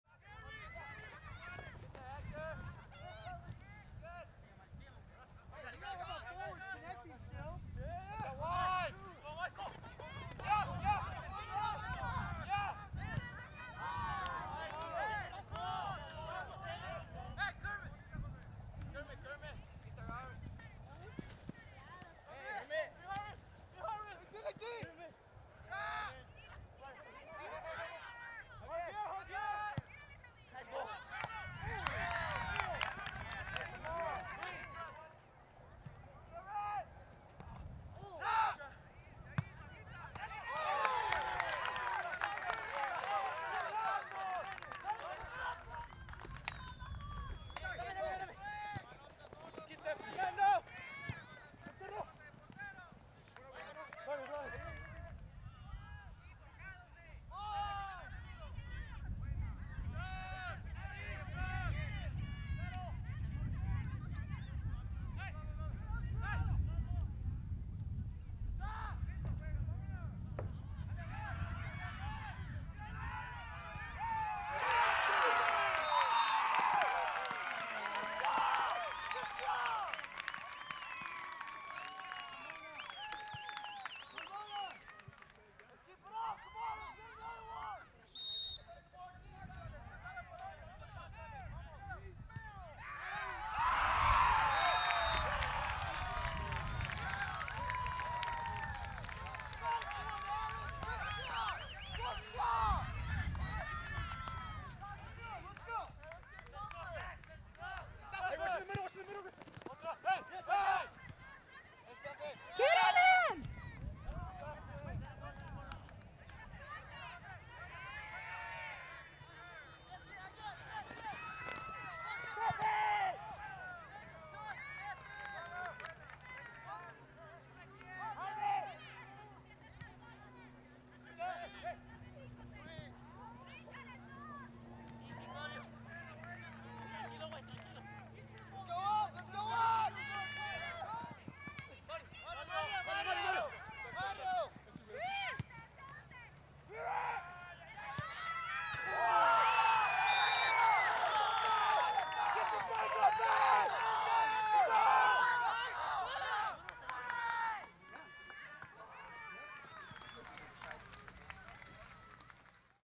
ScoreboardShow.tv Extra: Soccer Dodge City @ Great Bend, KS
Extra Highlights
High School Soccer
Dodge City @ Great Bend
September 20, 2012